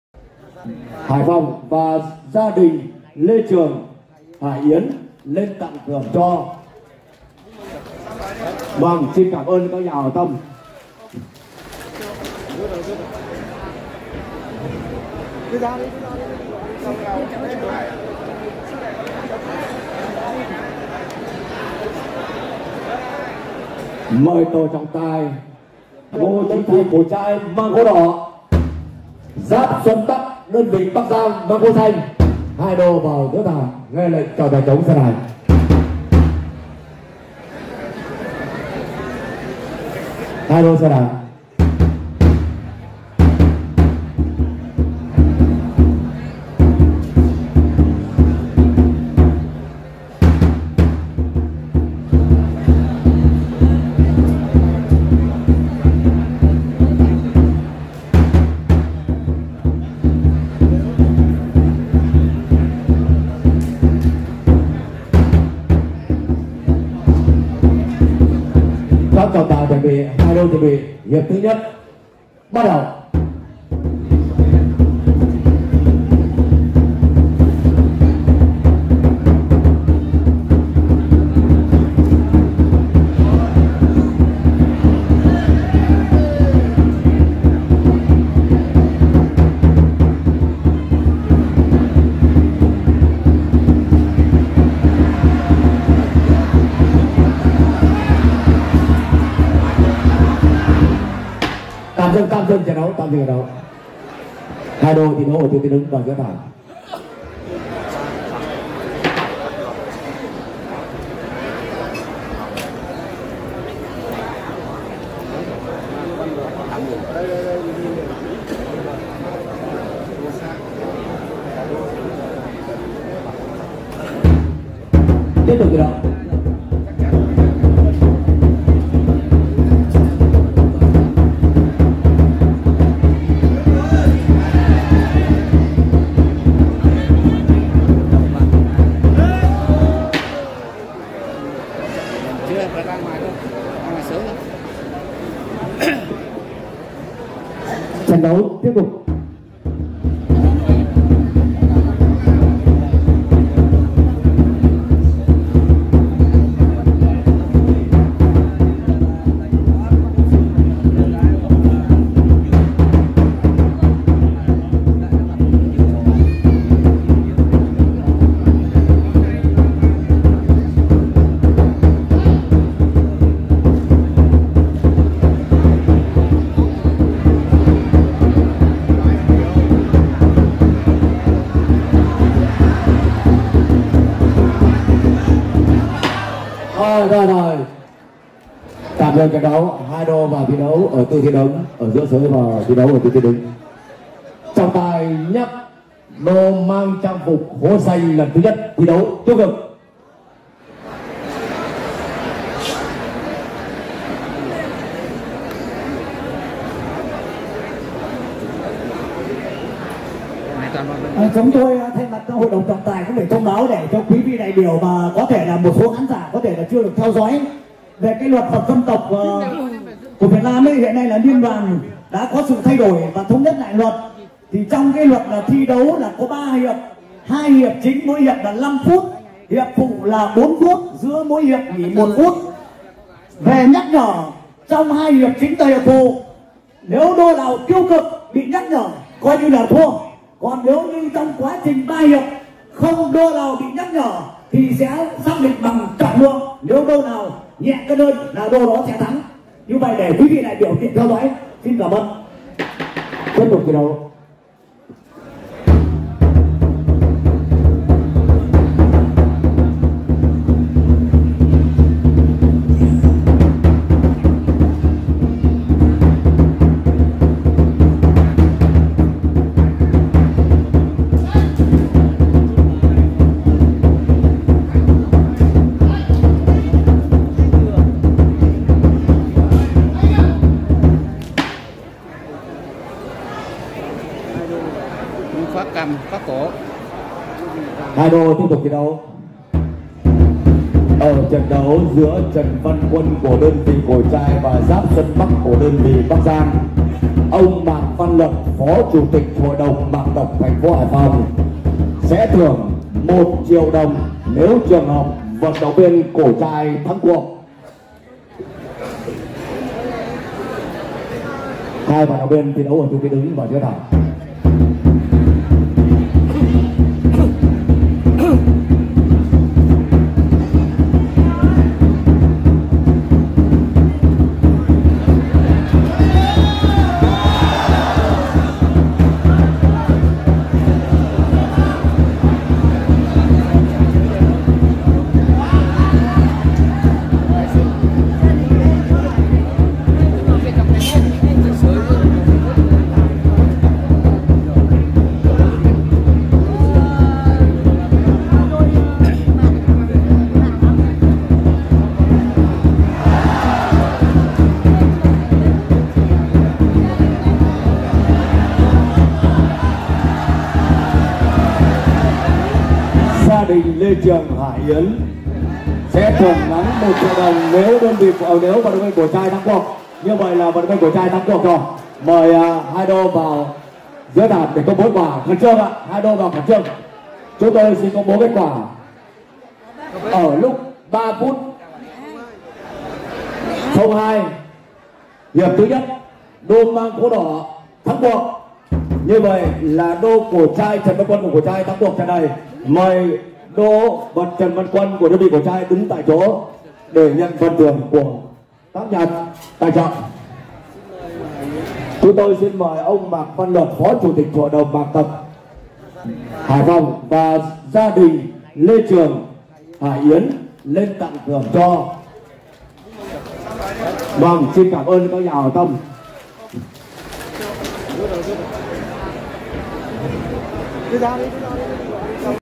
Nữ khán giả trao thưởng đô vật Hải Phòng thắng trận vật Hội thi vật nhà Mạc lần 3
Hàng ngàn du khách thập phương và người dân đã đổ về Từ đường họ Mạc Cổ Trai (xã Ngũ Đoan, huyện Kiến Thụy, TP. Hải Phòng) để xem Hội thi truyền thống Vật dân tộc thời Mạc lần thứ 3 năm 2024.

Hội thi được tổ chức nhằm mục đích không ngừng động viên, thúc đẩy phong trào truyền dạy, tập luyện môn vật dân tộc trong đông đảo nhân dân, nhằm bảo tồn và phát huy truyền thống văn hóa - thể thao của dân tộc, đồng thời khôi phục và phát huy các giá trị văn hóa dân gian đặc sắc của người dân trên vùng đất Dương Kinh xưa, tri ân Thái Tổ Nhân Minh Cao Hoàng Đế Mạc Đăng Dung là Đô lực sỹ - Võ Trạng nguyên, người có công khai sáng Vương triều Mạc.
Năm nay, hội thi diễn ra từ ngày 20 - 22/2 (tức ngày 11 - 13 tháng Giêng năm Giáp Thìn), thu hút câc đô vật đến từ các đoàn trong cả nước tham dự như: Hà Nam, Vũng Tàu, Lâm Đồng, Bắc Giang, TP. Hồ Chí Minh, Huế…, tranh tài ở 10 hạng cân. Trong đó, có 8 hạng cân dành cho nam với 45 đô nam và 2 hạng cân dành cho nữ.

Đây là hội thi vô cùng đặc sắc, chuyên nghiệp, được thực hiện theo Điều lệ của Liên đoàn Vật Việt Nam, qua đó quảng bá được bộ môn thể thao truyền thống của dân tộc, thu hút du khách đến với thành phố trong dịp xuân mới.